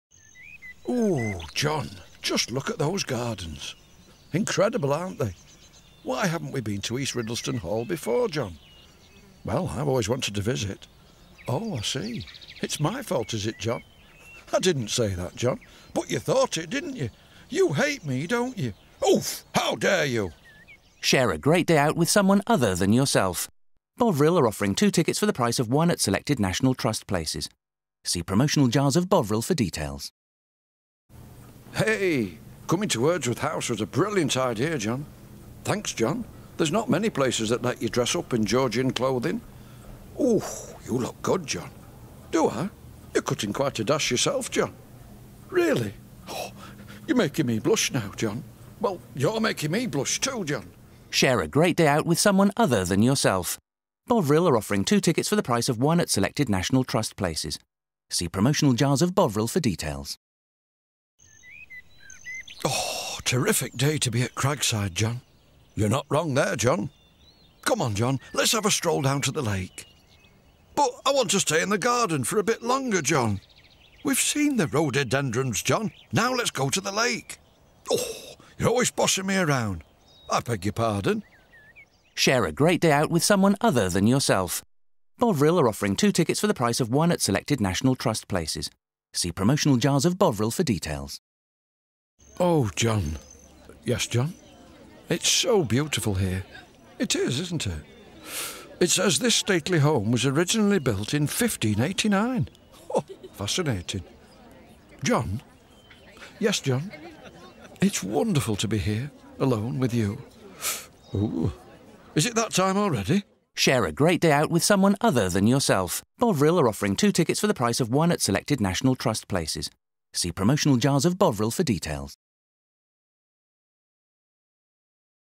Bovril radio ads
Bovril were offering a 2 for 1 entrance at National Trust sites. We used the comedy stylings of the legendary John Shuttleworth to make some smashing radio ads. Ooof!